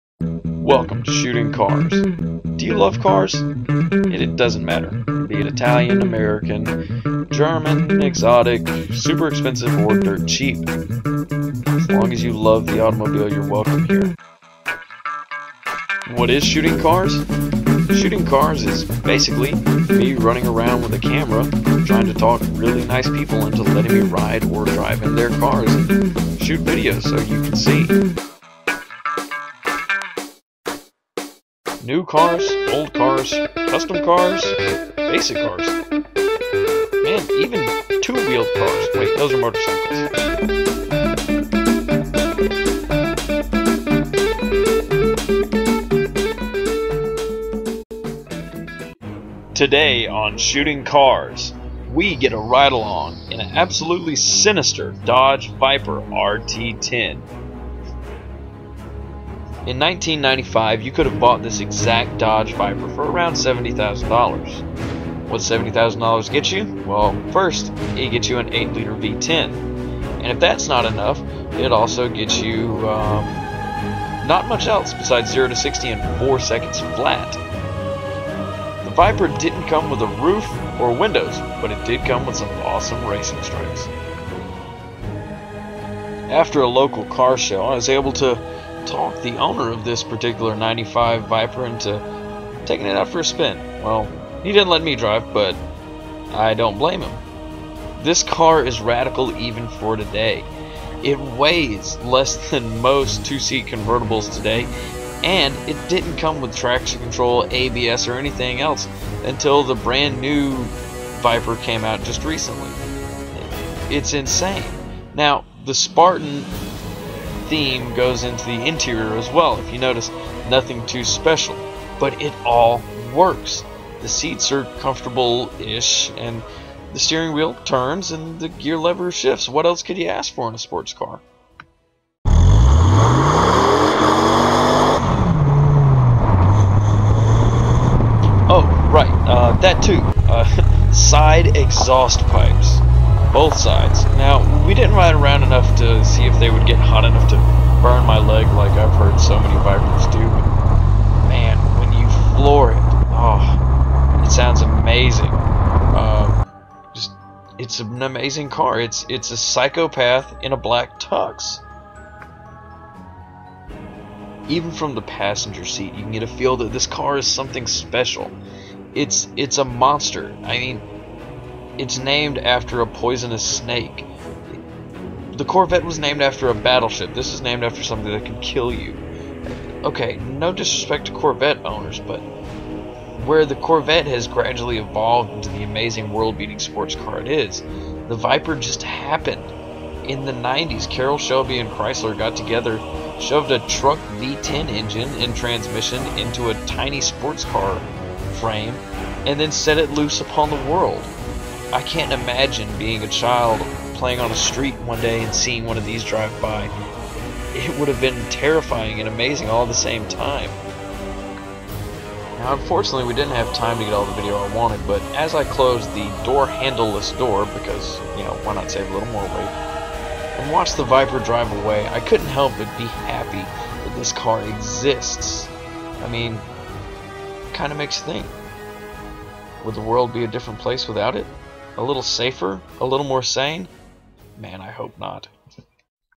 Shooting Cars 1-1 1995 Dodge Viper
This is just a quick peek at something I've been working on/dreaming of doing. I call it shooting cars and to sum it all up it's pretty much me, a GoPro and people with all different cars, doesn't matter what kind or how nice, just people and their cars. Oh and me.